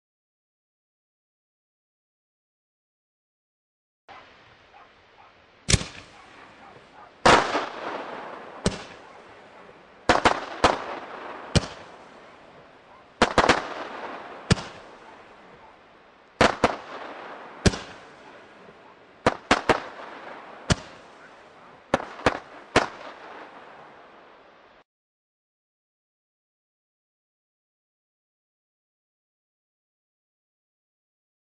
Kometenrohr Blau Crossette (368)
Bombenrohr,cal. 38 mm, 6 Schuss
Blau-goldene Crossette-Bouquets
Brenndauer ca. 20 Sec.